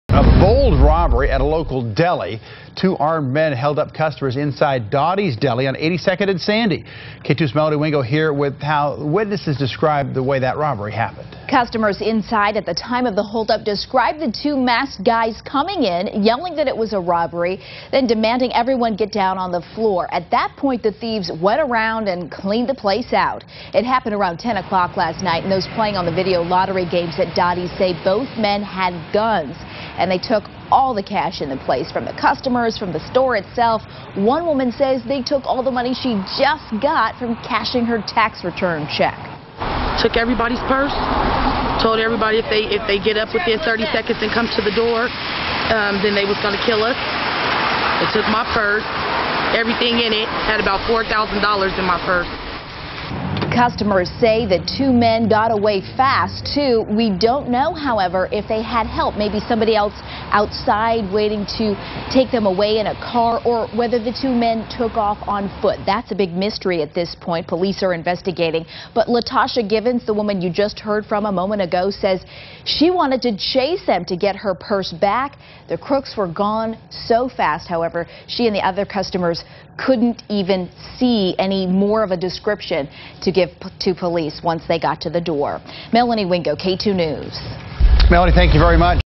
Robbers clean out customers in deli heist
PORTLAND, Ore. - Two armed men held up customers inside the Dottys Deli at about 10 p.m. Sunday night.Police said the crime involved two masked gunmen who came in with guns and demanded everyone get down on the floor before taking all the cash in the business.One woman who was in the deli at the